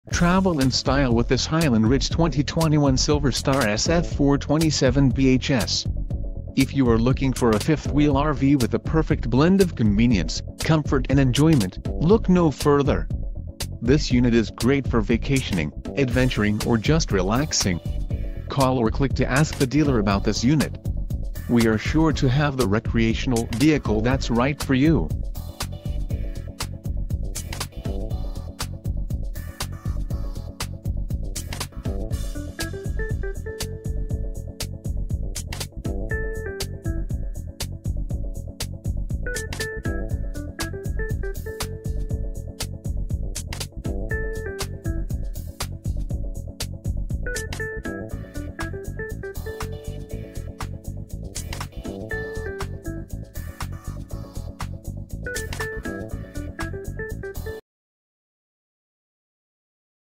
2021 Highland Ridge Silverstar SF427BHS - CCRV, LLC - Cor...
Highland Ridge Silverstar fifth wheel SF427BHS highlights: Bunkhouse Suite Dual Entry Full and Half Bath Queen Bed Dual Recliners Outdoor Kitchen   Imagine your family of six or more camping at your favorite campgrounds with your friends.  As the kids run around the campsite, you can easily set up the five slide outs, the two power awnings and get ready to relax or make a meal.  You will have full kitchen amenities with stainless steel appliances as well as an outside kitchen with griddle.  You might even like to choose the 18 or 21 cu. ft. refrigerator option, and/or add a washer/dryer option to the bedroom prepped space.  When inside everyone will have plenty of seating on the recliners, the theater seats and the free-standing dinette to be able to watch the 50 and quot; LED HD TV, to visit and to eat.